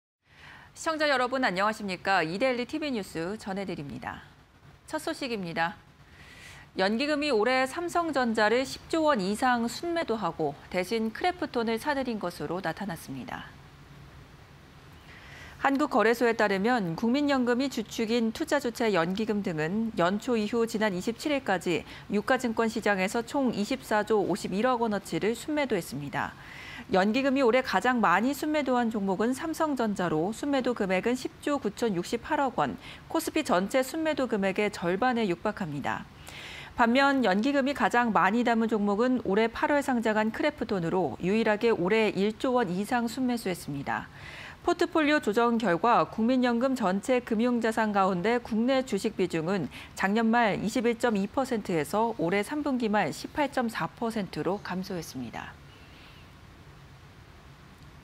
연기금, 올해 코스피 24조 순매도… 삼성전자 11조원어치 팔았다 (20211228)
연기금이 올해 삼성전자를 10조원 이상 순매도하고 대신 크래프톤, 삼성바이오로직스 등을 사들인 것으로 나타났습니다.
한국거래소에 따르면 국민연금이 주축인 투자 주체 '연기금 등'은 연초 이후 지난 27일까지 유가증권시장에서 총 24조51억원어치를 순매도했습니다.
연기금이 올해 가장 많이 순매도한 종목은 삼성전자로 순매도 금액은 10조9천68억원, 코스피 전체 순매도 금액의 절반에 육박합니다.
반면 연기금이 가장 많이 담은 종목은 올해 8월 상장한 크래프톤으로 유일하게 올해 1조원이상 순매수했습니다.
포트폴리오 조정 결과 국민연금 전체 금융자산 중 국내 주식 비중은 작년 말 21.2%에서 올해 3분기 말 18.4%로 감소했습니다.